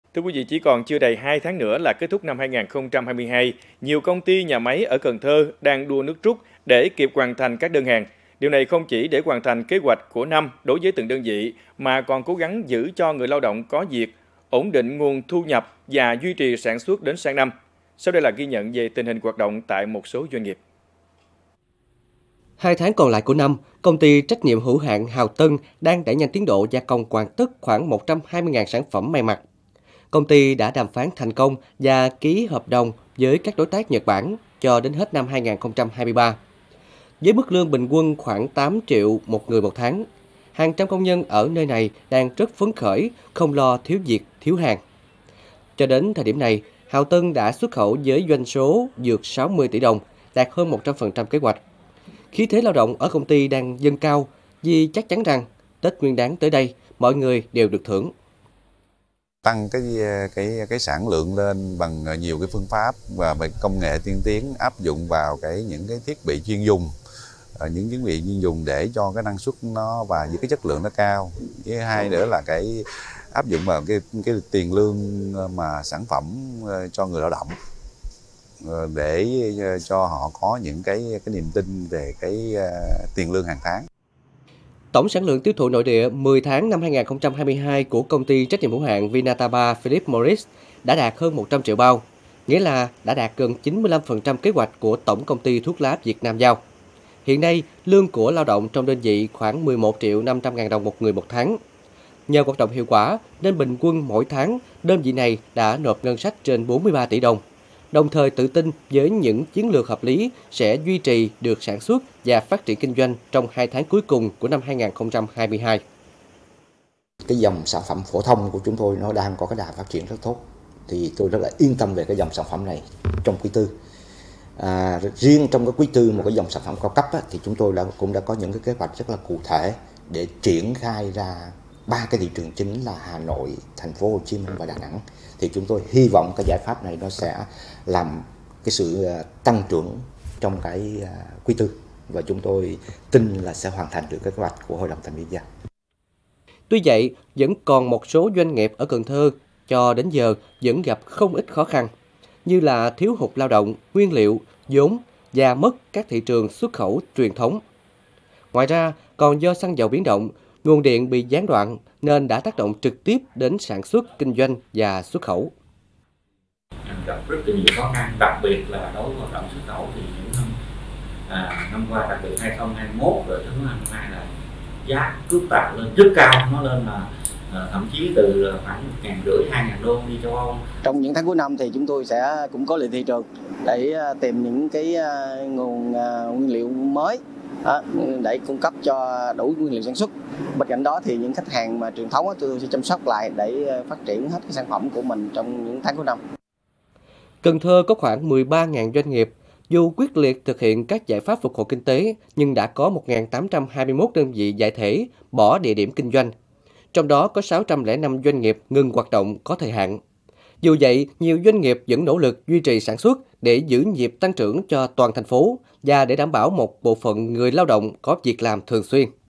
Doanh nghiệp Cần Thơ nỗ lực đạt chỉ tiêu năm 2022| Cần Thơ TV
Chỉ còn chưa đầy hai tháng nữa là kết thúc năm 2022. Nhiều công ty, nhà máy ở Cần Thơ đang đua nước rút để kịp hoàn thành các đơn hàng. Điều này không chỉ để hoàn thành kế hoạch của năm đối với từng đơn vị mà còn cố gắng giữ cho người lao động có việc, ổn định nguồn thu nhập và duy trì sản xuất đến sang năm. Sau đây là ghi nhận về tình hình hoạt động tại một số doanh nghiệp.